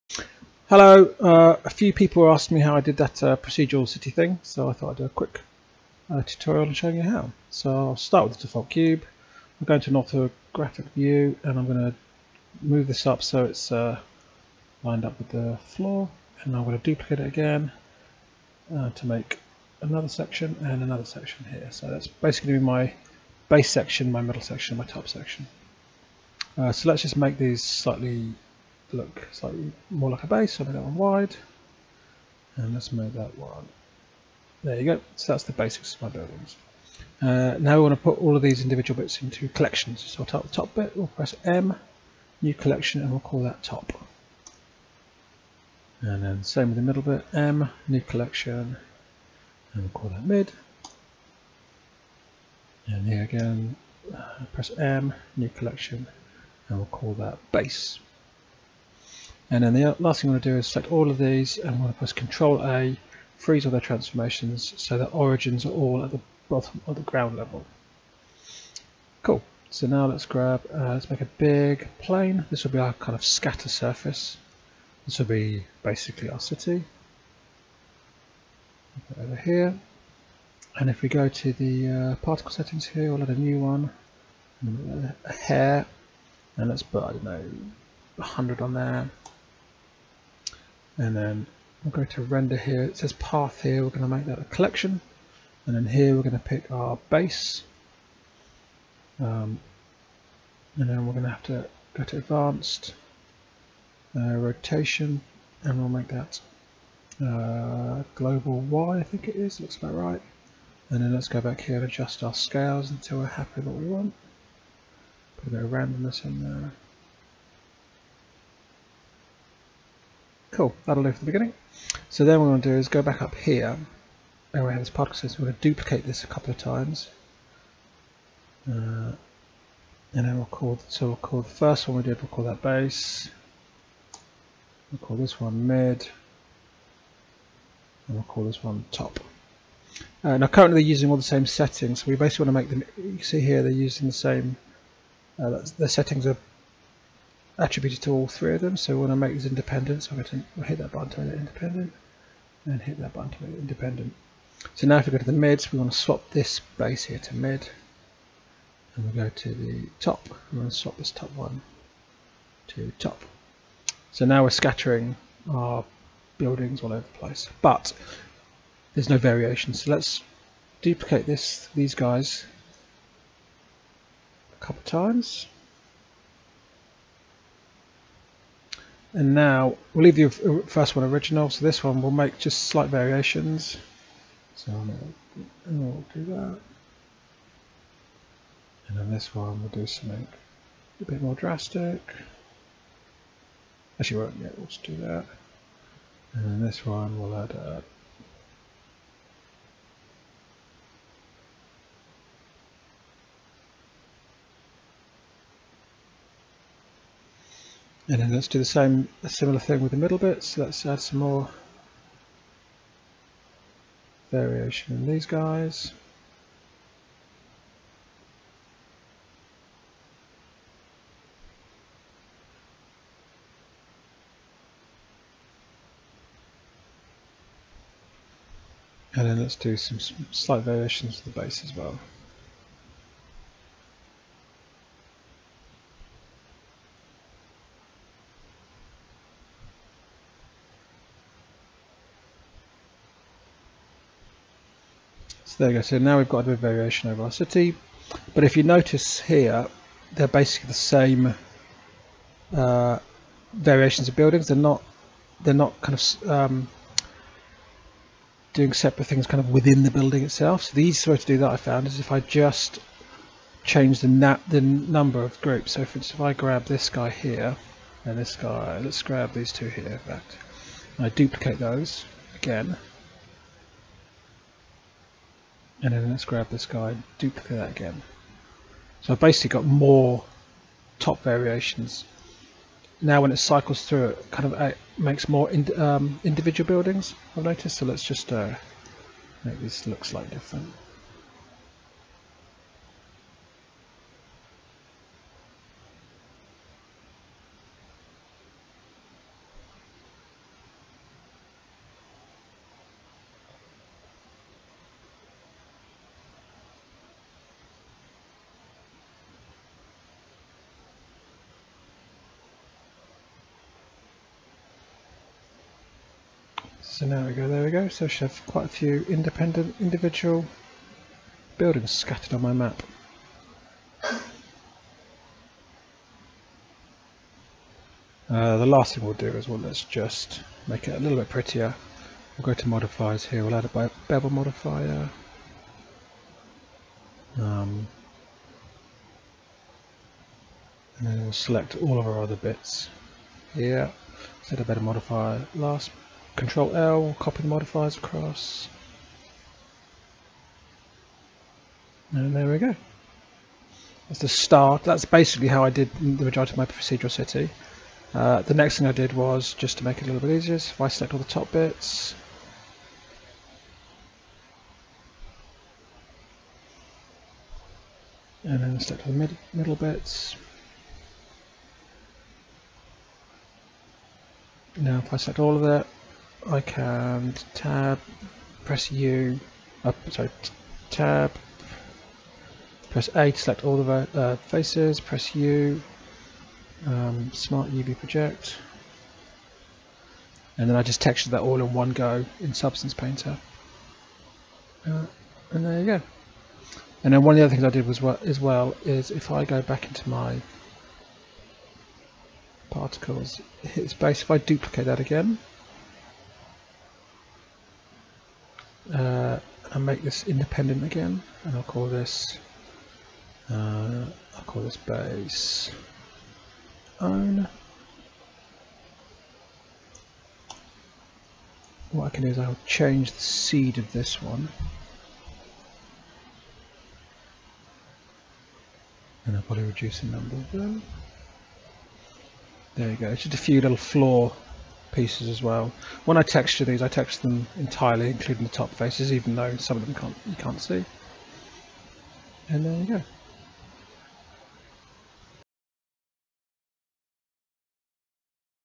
particle city, blender project
a quick city setup using hair particle systems in blender